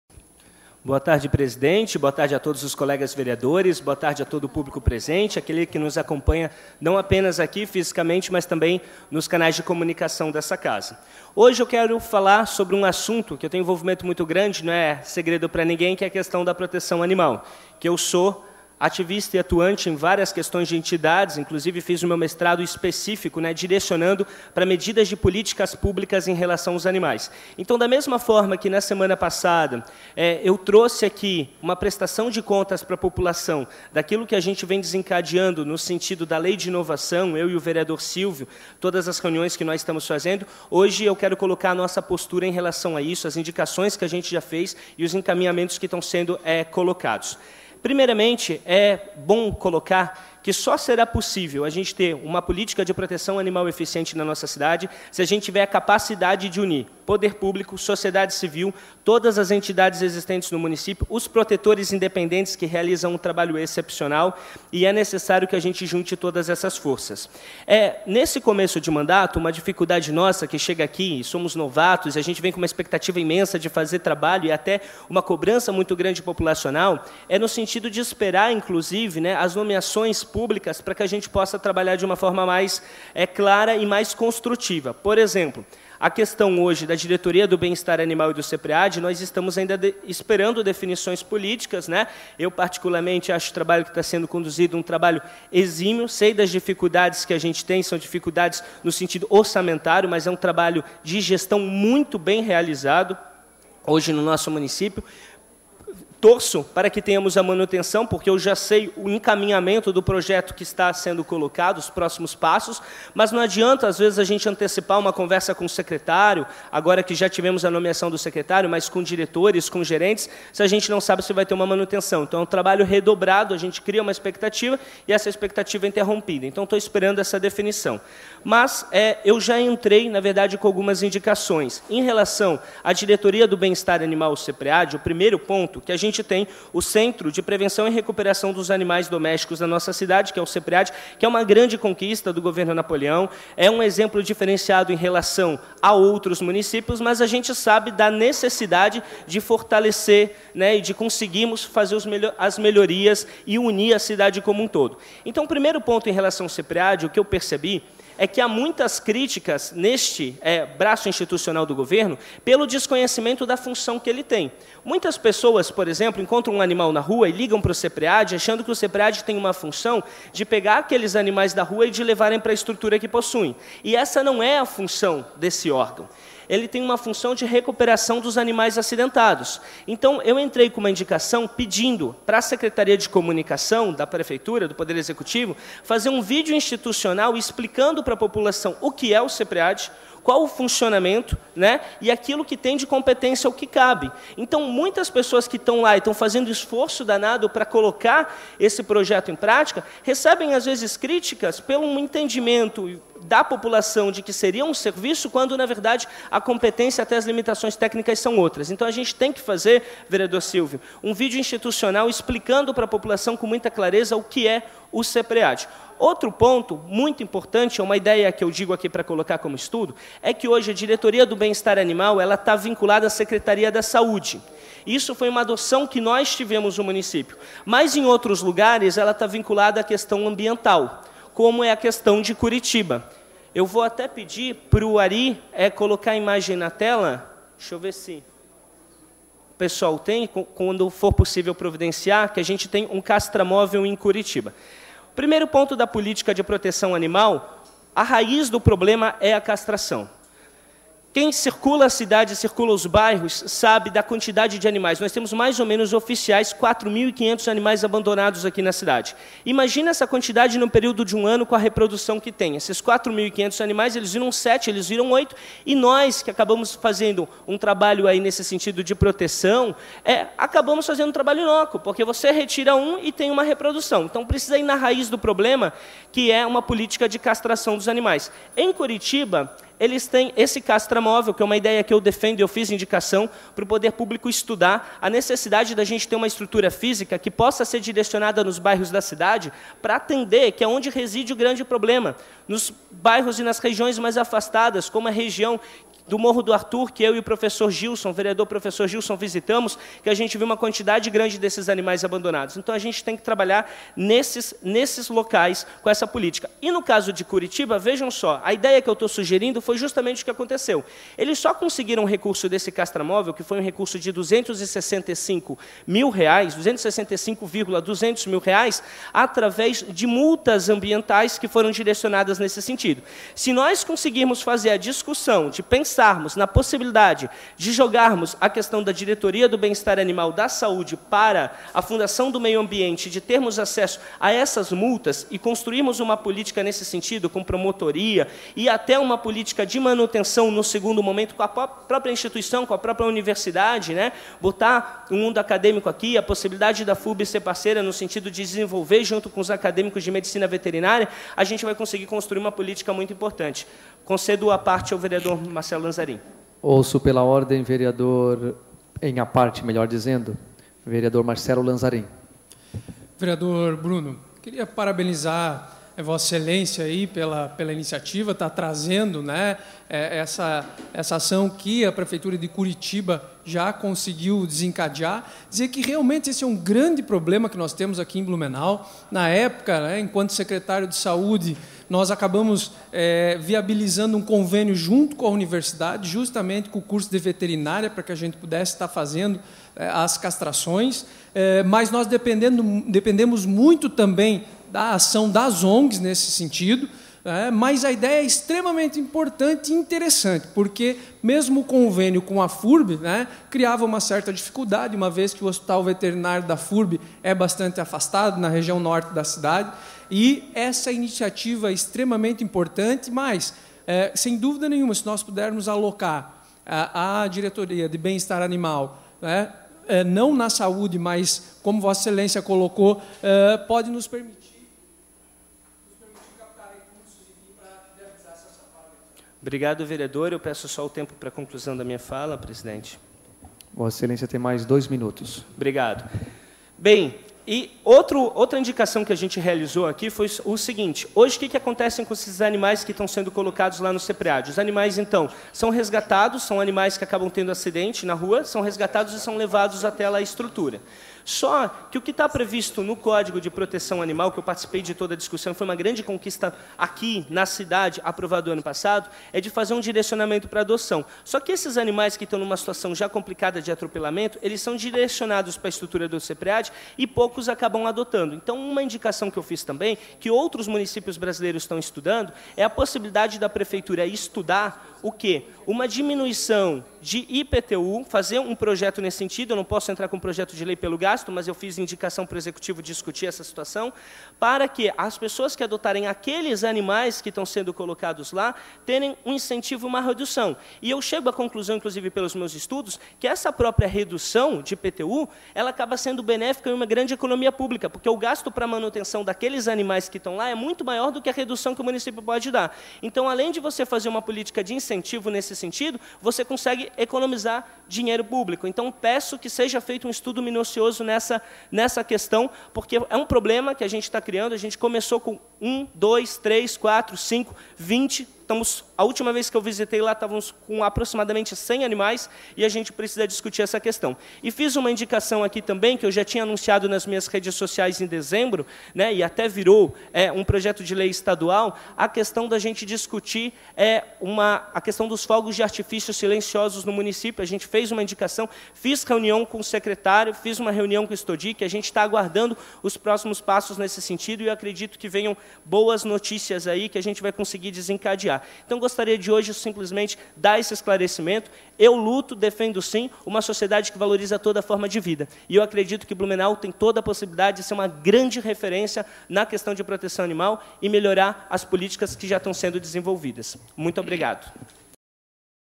Pronunciamento do Vereador Bruno Cunha 21/02/2017
Atualizado em 22/02 às 13h30.

O vereador Bruno Cunha (PSB) sugeriu que a Prefeitura faça um estudo aprofundado para avaliar a possibilidade de conceder desconto no IPTU para quem adotar animais recolhidos pelo CEPREAD (Centro de Prevenção e Recuperação de Animais Domésticos). Lembrou que Blumenau tem 4.500 animais abandonados. Comentou que sugeriu à Secretaria de Comunicação da Prefeitura que crie um vídeo institucional para explicar corretamente o papel do CEPREAD. Lembrou que muitas pessoas acham que o Centro tem por obrigação recolher todos animais abandonados. Disse que o órgão só recolhe animais em situação de risco ou que sofreram maus tratos. Falou ainda sobre a demora para nomeação da nova diretoria do órgão e que torce para manutenção dos atuais coordenadores. Salientou que é preciso criar também uma política de castração na cidade. Falou que Curitiba serve como exemplo para Blumenau, pois lá existe o Castra-Móvel, um veículo equipado com centro cirúrgico, com o qual se pretende esterilizar uma média de 500 animais por mês, entre cães e gatos abandonados ou que pertençam a famílias que não tenham condições financeiras de pagar pela castração.